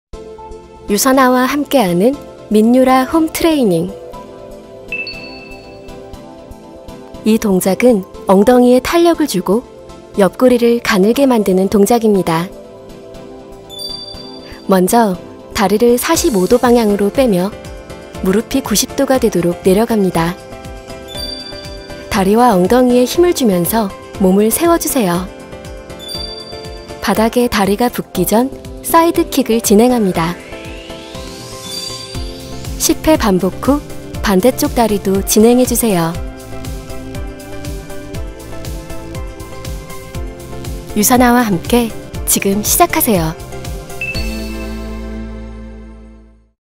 유사나와 함께하는 민유라 홈트레이닝 - 런지앤킥(Lunge&Kick)
유사나 28일 다이어트 챌린지
민유라 홈트레이닝 '런지앤킥'편

런지앤킥은 하체와 함께 옆구리 운동까지 할 수 있는 동작입니다.
처음부터 무리하기보다는 몸의 상태를 잘 확인하며 천천히 반복해보세요!